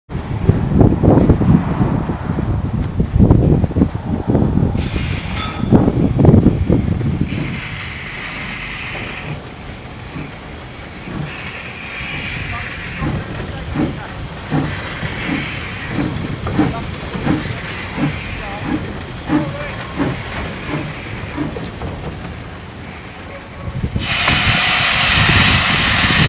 "Horden" shunting "Sir Cecil A Cochrane" out of Marley Hill shed
AB 0-6-0 "Horden" shunting RSH 0-4-0 "Sir Cecil A Cochrane out of Marley Hill shed on the Tanfield Railway